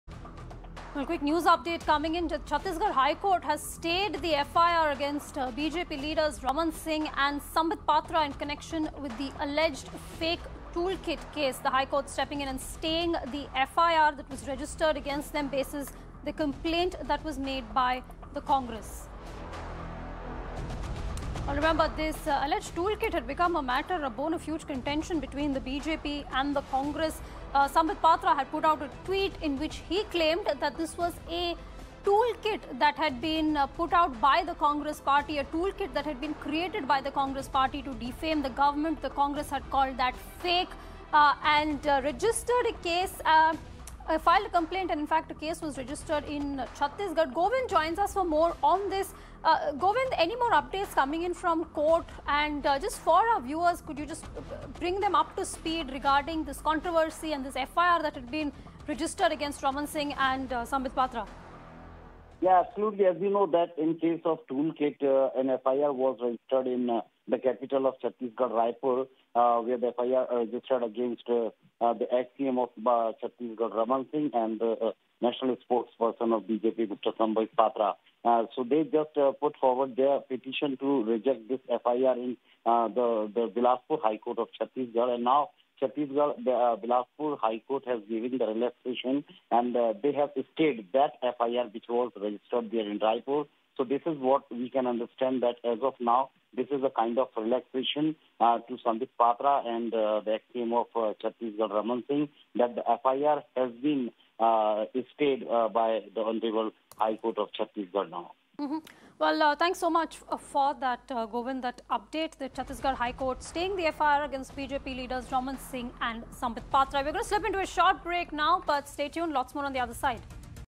Chhattisgarh HC stayed FIR order against Sambit Patra & Raman Singh in the alleged toolkit case
The Chhattisgarh High Court has stayed the FIR filed against BJP leaders Sambit Patra and Raman Singh in the alleged toolkit Case. The toolkit became a bone of contention between the two parties when BJP leader Sambit Patra put out a tweet, claiming that the toolkit was framed by the Congress party in order to defame the Centre in its efforts. The Congress party registered a case in the Chhattisgarh HC on May 18, following which the High Court termed it a manipulated media. A petition was filed by the accused BJP leaders in the Bilaspur High Court which was later rejected.